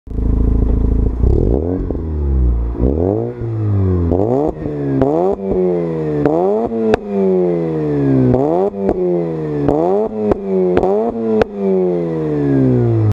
RX8 toyosport sportcat 200cell
07' Mazda RX8 192hp
-K&N air filter
-Toyosport cat 200 cellules
-Japspeed cat-back
-Brand new OEM sparks, wires & coils